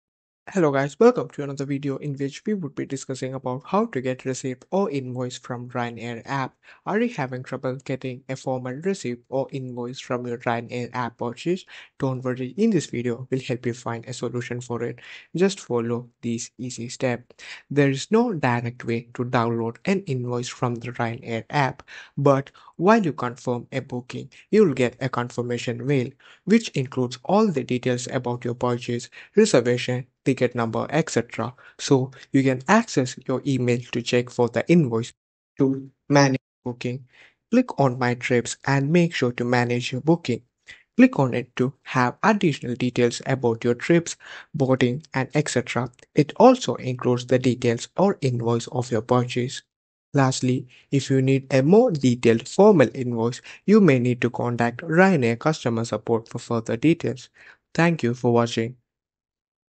How To Get Receipt From Ryanair App (How To Get Invoice On Ryanair)
Need An Invoice For Your Flight? This Video Will Walk You Through How To Retrieve Your Receipt On The Ryanair App.